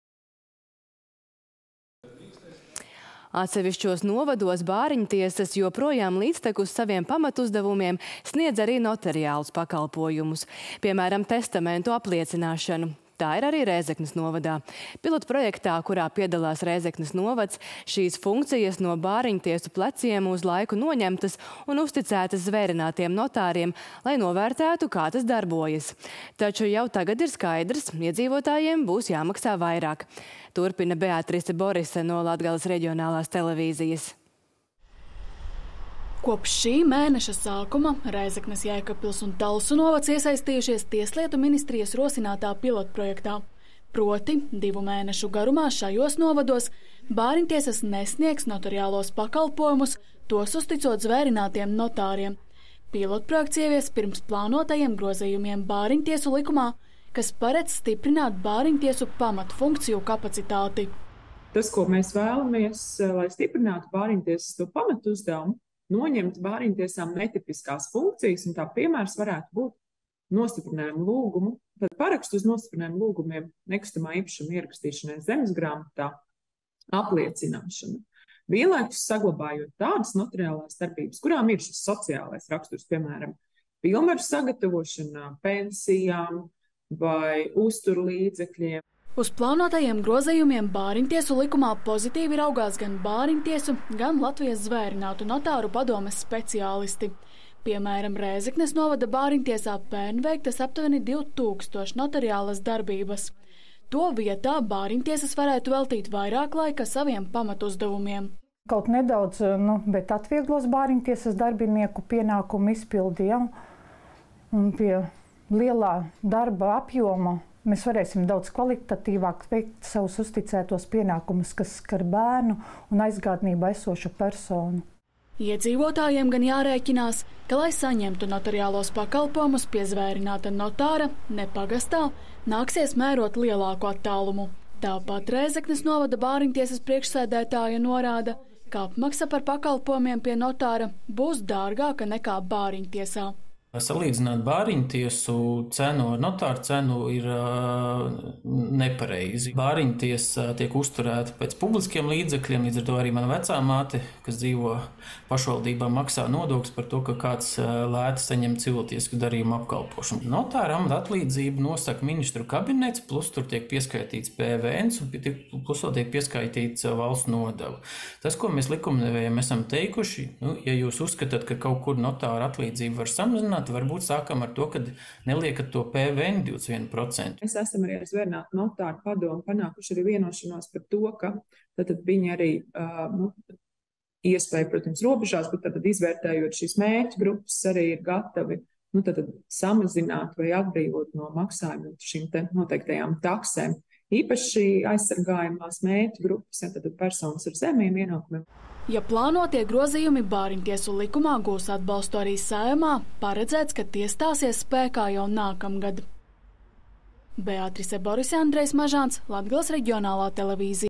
Bāriņtiesām piedāvā vairs neveikt notariālas funkcijas
Atsevišķos novados bāriņtiesas joprojām līdztekus saviem pamatuzdevumiem sniedz arī notariālus pakalpojumus. Piemēram, testamentu apliecināšanu. Tā ir arī Rēzeknes novadā. Pilotprojektā, kurā piedalās Rēzeknes novads, šīs funkcijas no bāriņtiesu pleciem uz laiku noņemtas un uzticētas zvērinātiem notāriem, lai novērtētu, kā tas darbojas. Bet jau tagad ir skaidrs – iedzīvotājiem būs jāmaksā vairāk. Turpina Beatrise Borise no Latgales reģionālās televīzijas.